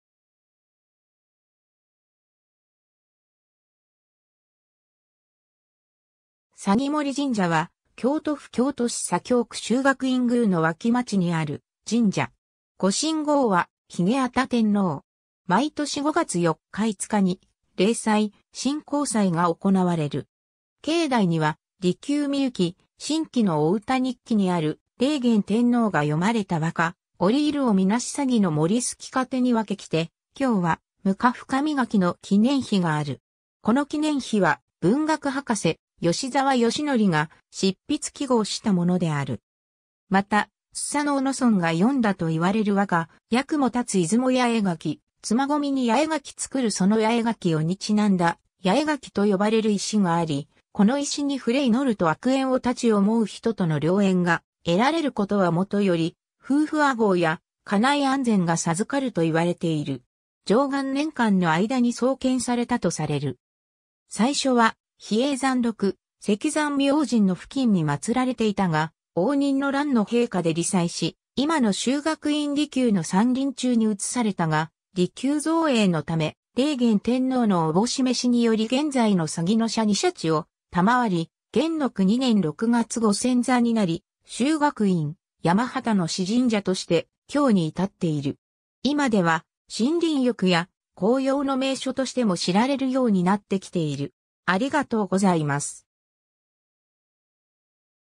鷺森神社
鷺森神社（さぎもりじんじゃ、さぎのもりじんじゃ）は京都府京都市左京区修学院宮ノ脇町にある神社。
御神号は鬚咫天王（すだてんのう、しゅだてんのう）。
 毎年5月4日・5日に例祭「神幸祭」（さんよれ祭）が行われる。
境内には離宮御幸震記の御歌日記にある霊元天皇が詠まれた和歌「をりゐるを 見し鷺の森 すきかてに わけきて今日は むかふ神垣」の記念碑がある。
この記念碑は文学博士吉沢義則が執筆揮毫したものである。
また、素盞嗚尊が詠んだと言われる和歌「八雲立つ 出雲八重垣 妻籠みに 八重垣作る その八重垣を」にちなんだ「八重垣」と呼ばれる石があり、この石に触れ祈ると悪縁を絶ち思う人との良縁が得られることはもとより、夫婦和合や家内安全が授かると言われている。
 貞観年間（859年 - 877年）の間に創建されたとされる。
最初は比叡山麓、赤山明神の付近に祀られていたが応仁の乱の兵火で罹災し、今の修学院離宮の山林中に移されたが、離宮造営の為、霊元天皇の思召しにより現在の鷺の社に社地を賜り、元禄2年6月御遷座になり修学院、山端の氏神社として今日に至っている。
 今では森林浴や紅葉の名所としても知られるようになってきている。